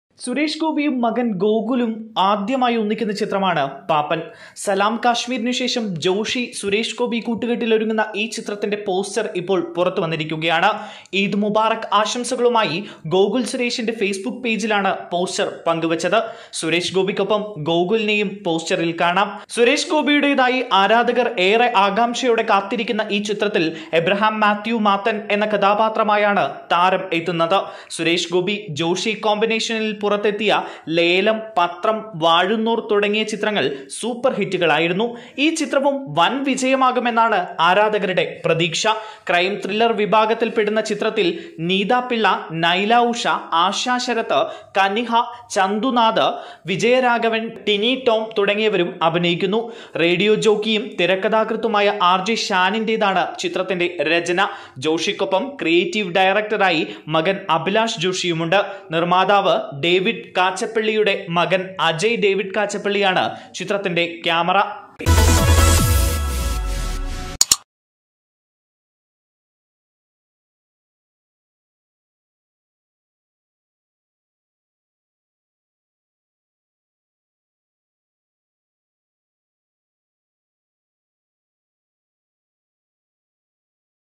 മകനൊപ്പം സുരേഷ് ഗോപി, ജോഷി ചിത്രം പാപ്പന്റെ പോസ്റ്റര്‍ പുറത്ത് | Paappan |Suresh Gopi
സുരേഷ് ഗോപിയും മകന്‍ ഗോകുലും ആദ്യമായി ഒന്നിക്കുന്ന ചിത്രമാണ് 'പാപ്പന്‍'. സലാം കാശ്മീരിനു ശേഷം ജോഷി-സുരേഷ് ഗോപി കൂട്ടുകെട്ടിലൊരുങ്ങുന്ന ഈ ചിത്രത്തിന്റെ പോസ്റ്റര്‍ പുറത്ത് വിട്ടിരിക്കുകയാണ്. ഈദ് മുബാറക്ക് ആശംസകളുമായി ഗോകുല്‍ സുരേഷിന്റെ ഫേസ്ബുക്ക് പേജിലാണ് പോസ്റ്റര്‍ പങ്കുവെച്ചത്. സുരേഷ് ഗോപിക്കൊപ്പം ഗോകുലിനേയും പോസ്റ്ററില്‍ കാണാം. pappan sureshgopi gokulsuresh | Pappan | Suresh Gopi | Gokul Suresh | Joshiy | David Kachappilly | Eid Mubarak